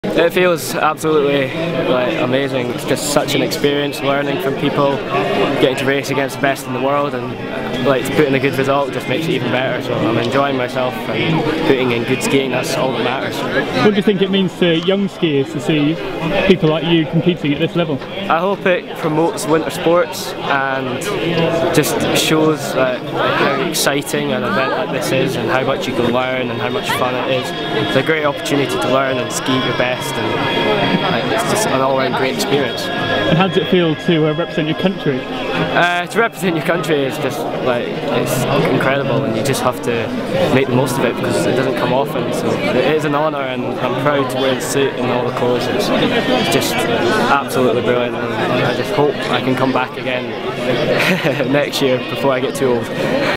Paul Henderson Interview - Innsbruck YOG 2012
Tom Mallion interviews Paul Henderson (TeamGB Super G) after competing at the first ever Youth Winter Olympic Games in Innsbruck, Austria 2012.